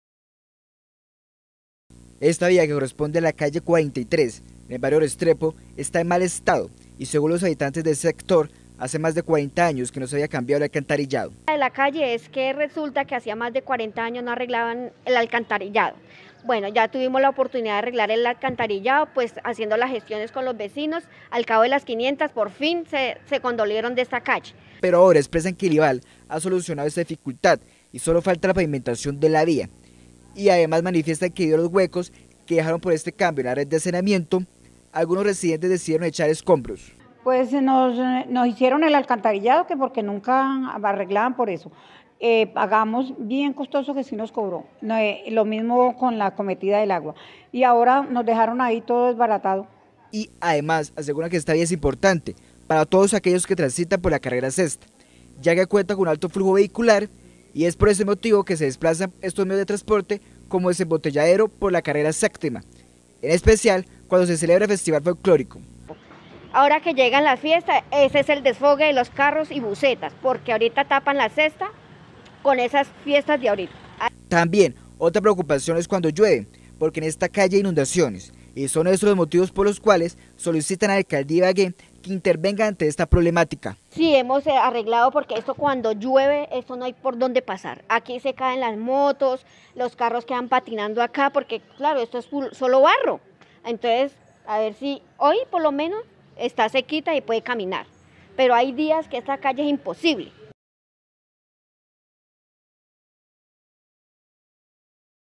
Vía clave para desembotellar la carrera sexta en época de fiestas no estará habilitada para junio.
El Ibal hace 10 años cabio el alcantarillado pero calle está sin pavimentar.